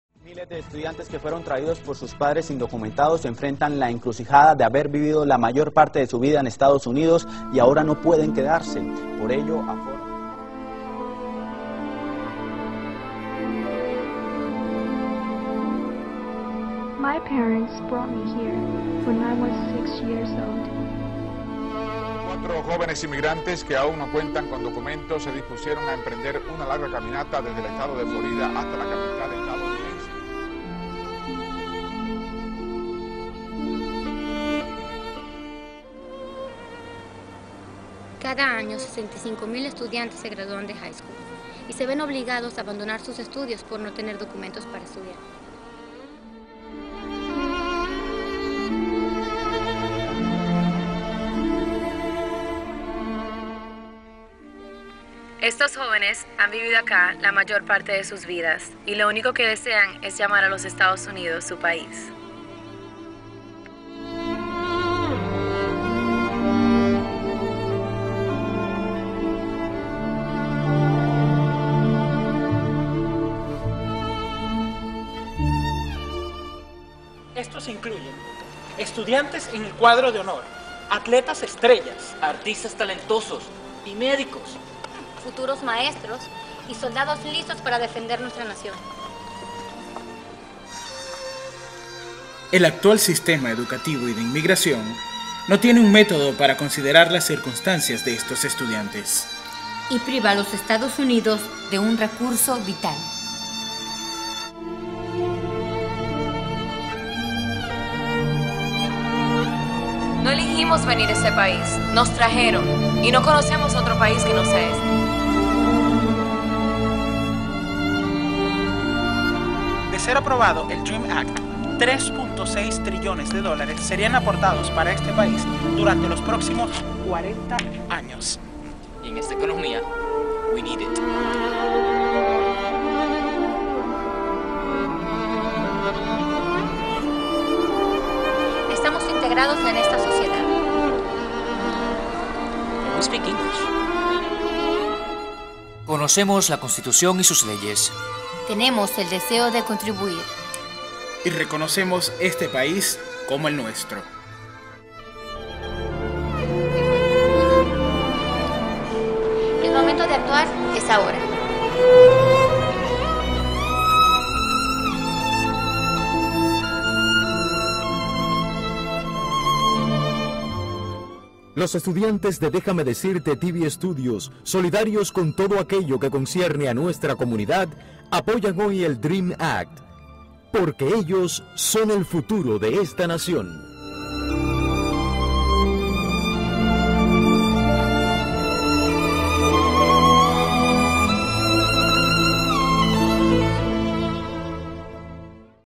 Dream Act
Los estudiantes de Dejame decirte estudios apoyan el Dream act.